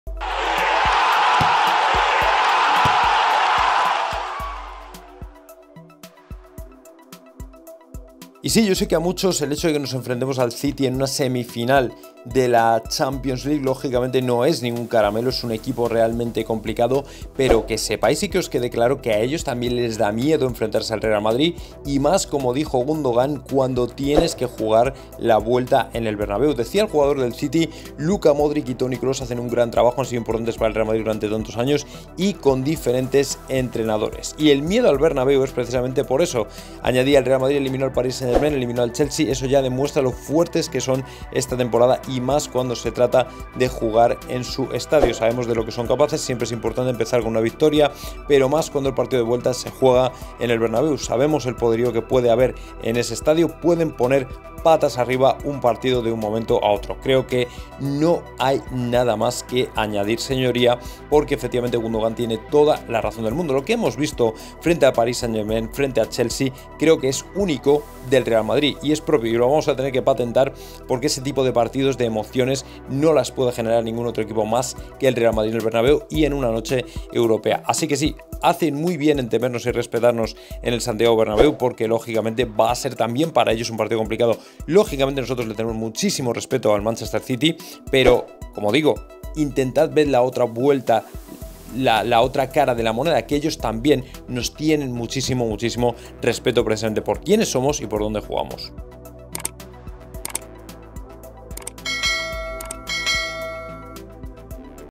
¡LES DAMOS MIEDO! | Gundogan SORPRENDE al madridismo al desvelar qué es lo que MÁS LES PREOCUPA
1. ¡Obtenga 1 camiseta gratis, ordene 3 artículos!
2. ¡Obtenga 2 camisetas gratis, ordene 5 artículos!